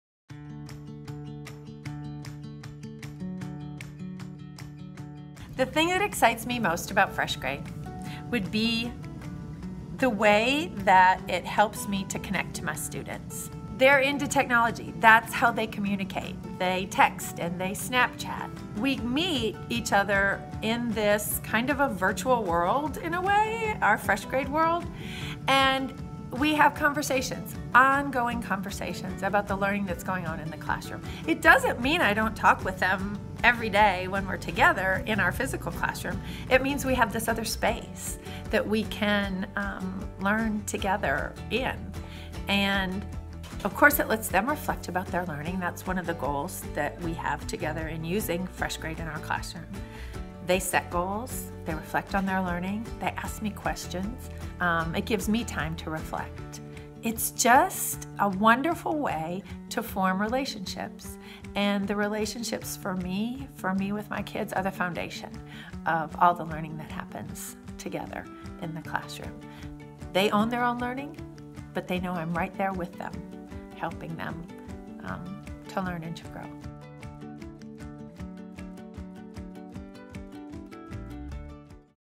Engaging in Ongoing Learning Conversations with Students
FreshGrade Next is a learning network that uses the same technology as the social networks people are familiar with today, but orients it to authentic student learning.

Kathy Rogers, a teacher at Pizitz Middle School in Vestavia Hills, Alabam explains how FreshGrade provides a space for her and her students to learn together and engages in ongoing conversations. With FreshGrade, students are prompted to own their learning through goal setting and self-reflection. Teachers can form relationships with students outside of the classroom and reflect on their teaching to guide students to their next step of success.